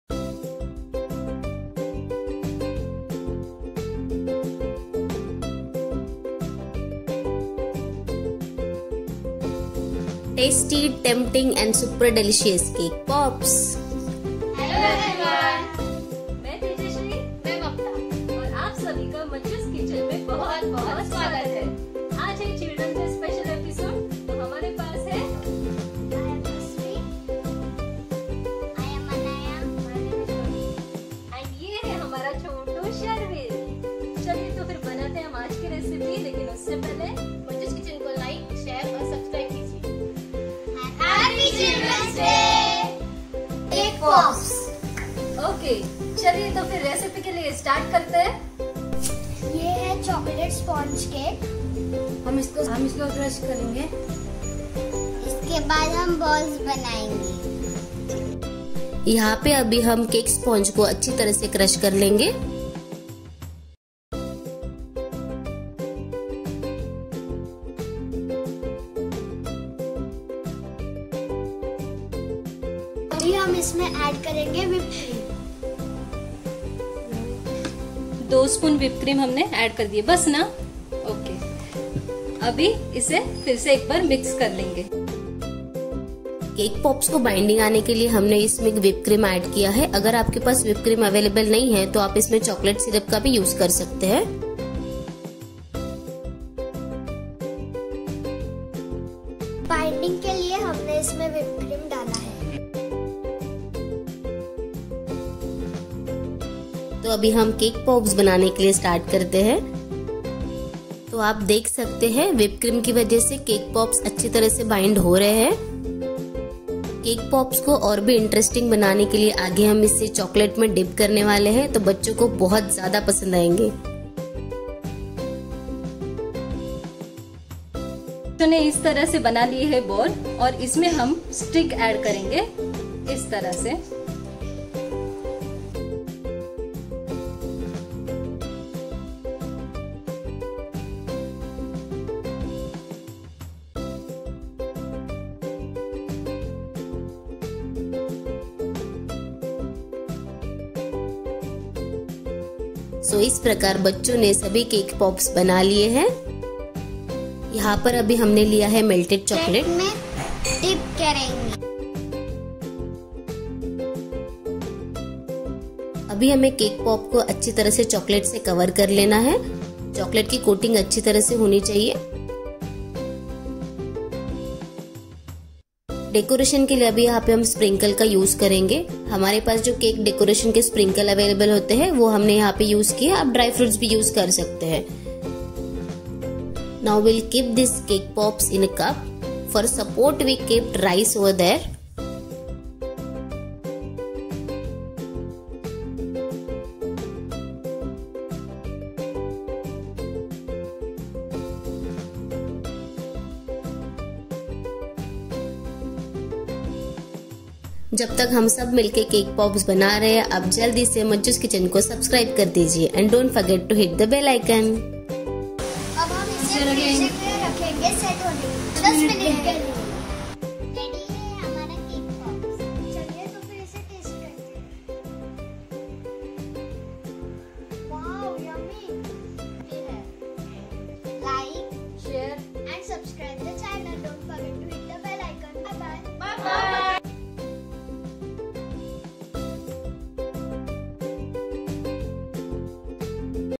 CAKE POP'S CHILDREN DAY SPECIAL |MAJJUS KITCHEN
Cake pops❤️

Hosted by : Mamta Dange Ingole

Cake pops are made of cake and chocolate coating.  It's very tempting and delicious

Ingredients
Cake sponge  1
Whipped cream 2 tbs
Sprinkle for decorating 1 tbs
Dark chocolate 1 cup

Method:
 First crushed the cake then add whipped cream in it for binding once done make small ball of crushed cake. Take melted chocolate and dipped cake pops in it.  Keep pops in fridge for 10 min.
Serve chilled and Enjoy with family ❤️